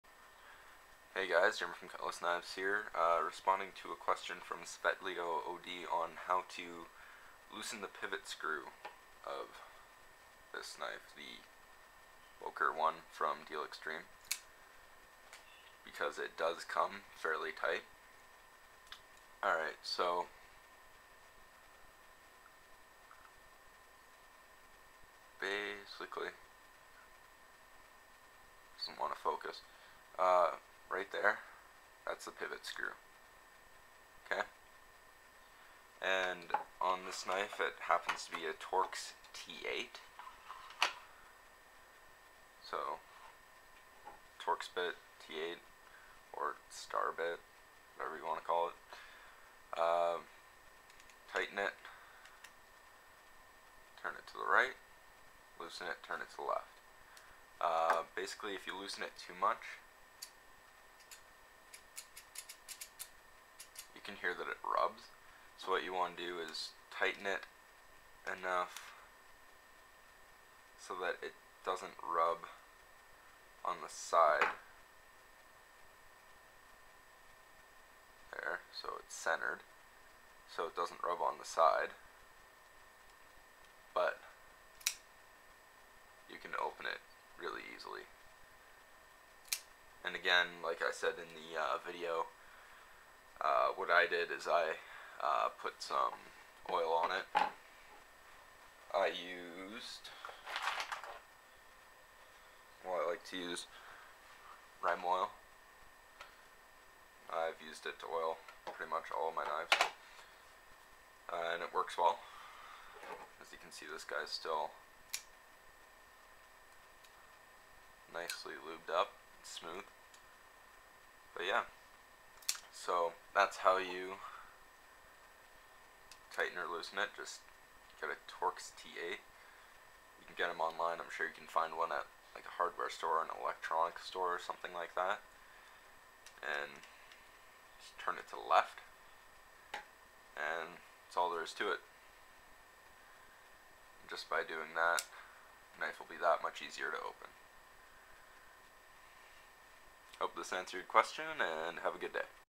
How to Loosen Pivot Screw
Showing how to loosen the Pivot Screw on the "Boker" knife from DealExtreme.